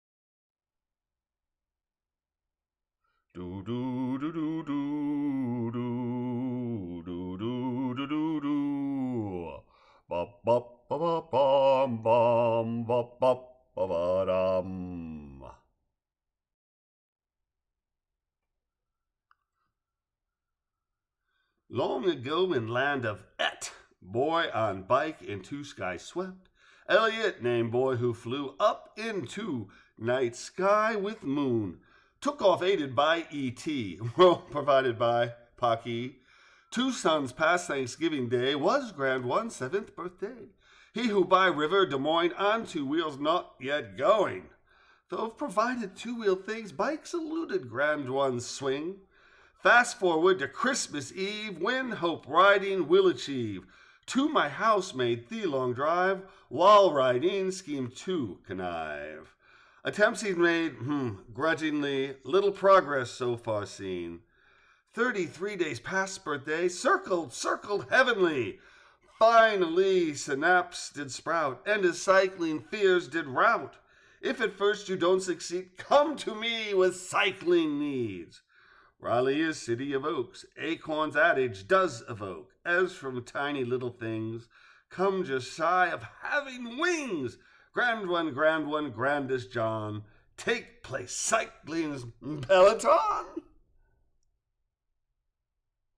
Finally Synapse Did Sprout!
An ode to my just turned seven-years-old Grandone who learned to ride a bicycle during his Christmas visit at my house.